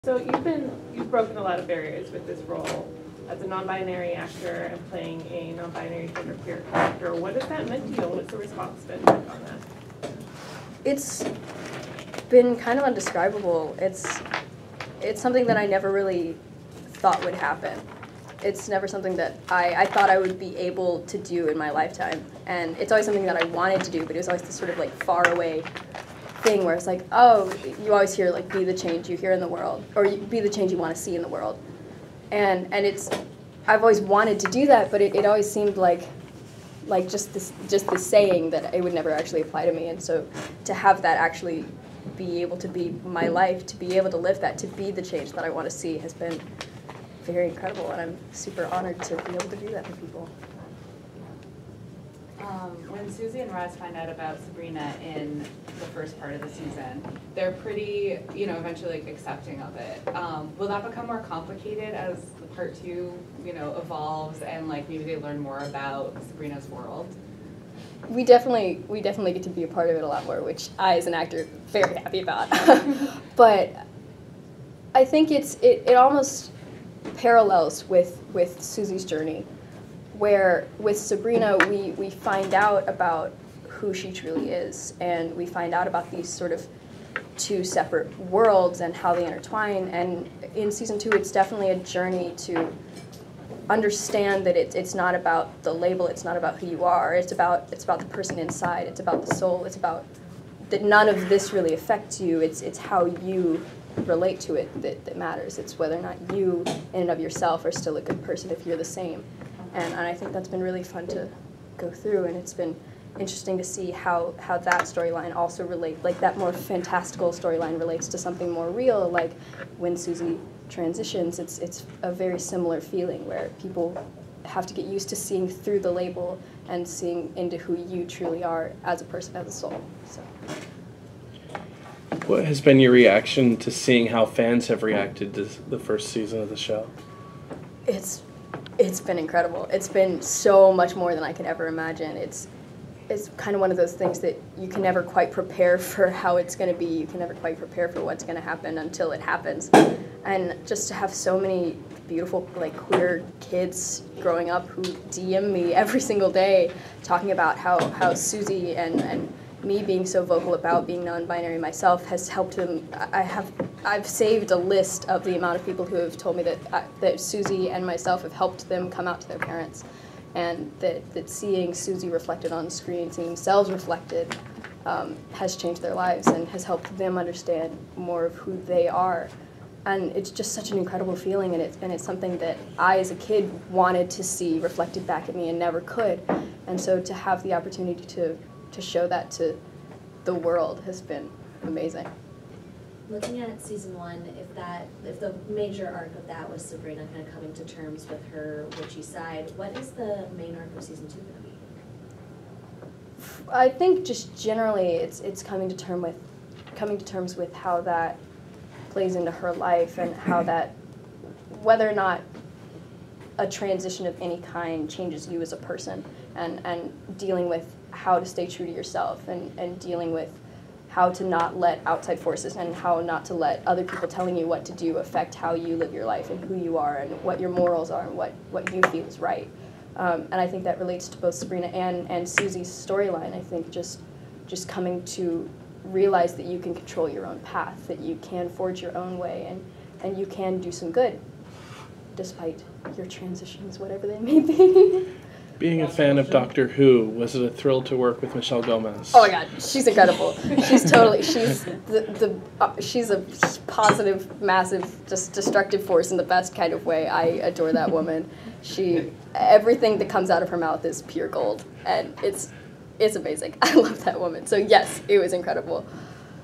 Lachlan Watson | Chilling Adventures of Sabrina Season 1B
Lachlan Watson talks about what's coming up for Susie/Theo in the second half of Season 1 (Season 1B) on the Netflix Original Series The Chilling Adventures of Sabrina. Please note that some minor SPOILERS for Season 1B are being discussed within. Also in this interview, Lachlan geeks out over their fan love for Michelle Gomez, and talks about the reaction they have gotten from the fans in playing such a groundbreaking role.

The Chilling Adventures of Sabrina Season 1B drops April 5, 2019 on Netflix.